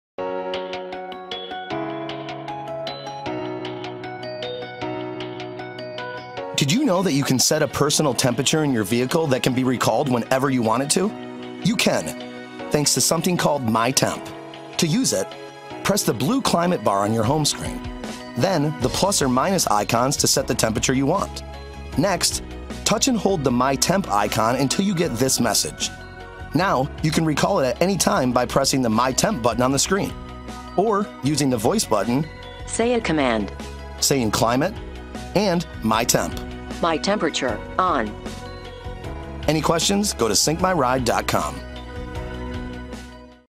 SYNC® with MyFord or MyLincoln Touch™ Personalizing Climate with MyTemp
Do you have a special temp that keeps you comfy in the car? SYNC can do that for you, learn how.